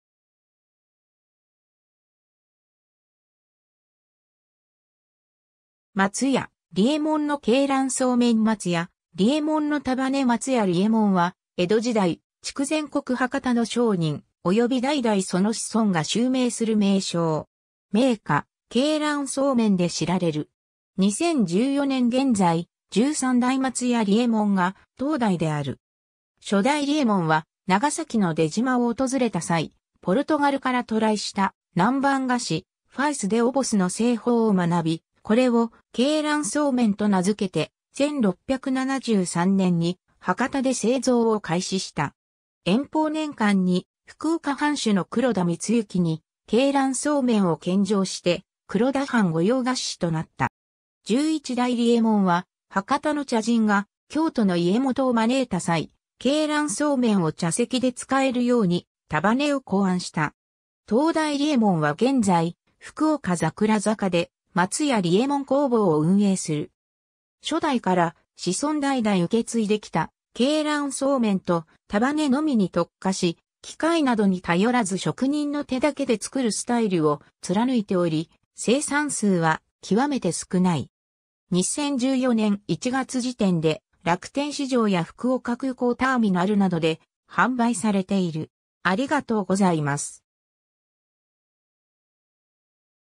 松屋利右衛門
松屋利右衛門の鶏卵素麺（けいらんそうめん） 松屋利右衛門のたばね 松屋利右衛門（まつやりえもん、初代:生年不明 - 1701年（元禄12年11月7日）は、江戸時代、筑前国（福岡県）博多の商人、および代々その子孫（後継者）が襲名する名称。
銘菓「鶏卵素麺（けいらんそうめん）」で知られる。
 2014年現在、十三代松屋利右衛門（1970年1月4日-）が当代である。
 初代利右衛門は長崎の出島を訪れた際、ポルトガルから渡来した南蛮菓子fios de ovos（フィオス・デ・オヴォス、卵の糸）の製法を学び、これを「鶏卵素麺」と名付けて、1673年（延宝元年）に博多で製造を開始した。
延宝年間に福岡藩主の黒田光之に鶏卵素麺を献上して、黒田藩御用菓子司となった。
 十一代利右衛門は博多の茶人が京都の家元を招いた際、鶏卵素麺を茶席で使えるように「たばね（鶏卵素麺を一口サイズに切って昆布でたばねた菓子）」を考案した。
 当代利右衛門は現在、福岡桜坂で松屋利右衛門工房を運営する。
初代から子孫代々受け継いできた「鶏卵素麺」と「たばね」のみに特化し、機械などに頼らず職人の手だけで作るスタイルを貫いており、生産数は極めて少ない。
2014年01月時点で、楽天市場や福岡空港ターミナルなどで販売されている。